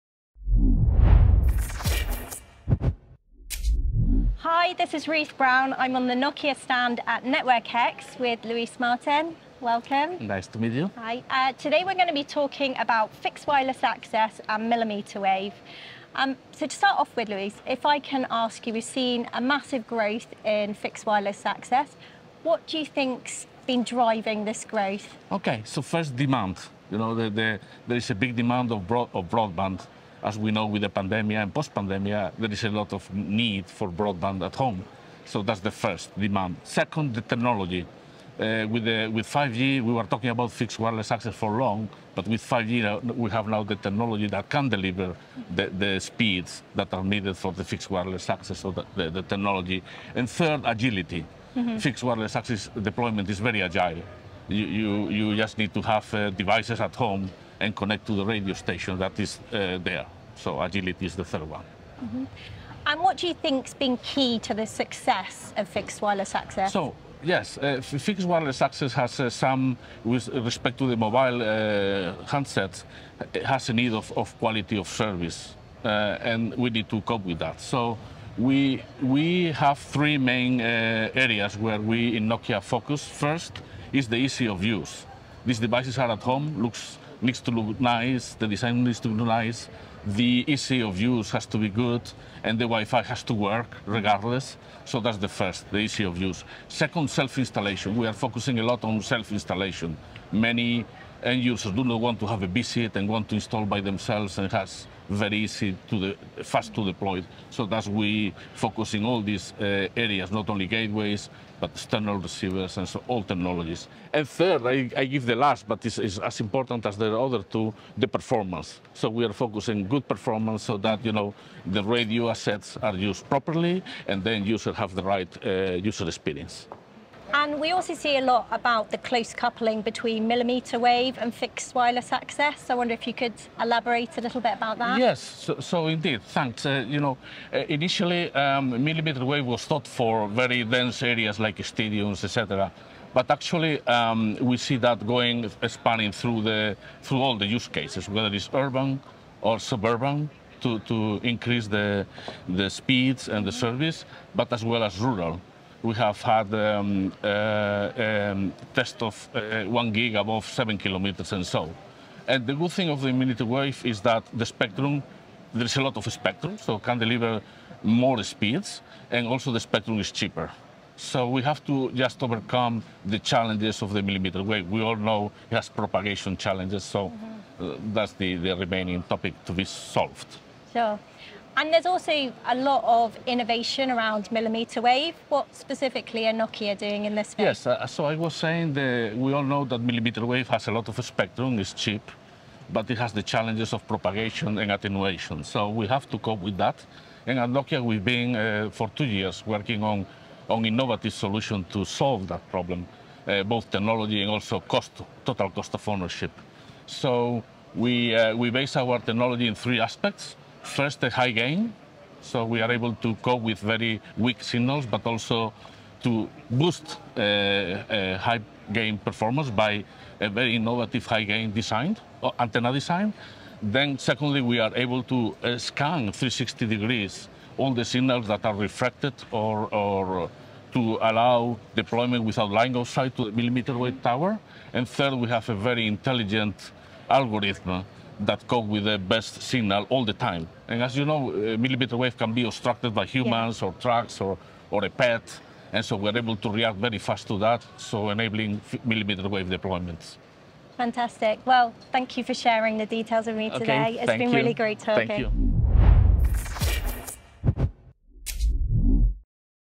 The rise of mmWave Fixed Wireless Access
New Nokia innovations now make it possible for 5G mmWave to be deployed in urban, suburban and rural use cases, at scale. Luis Martin Santiago, Head of FWA Business Line, Nokia Fixed Networks explains how.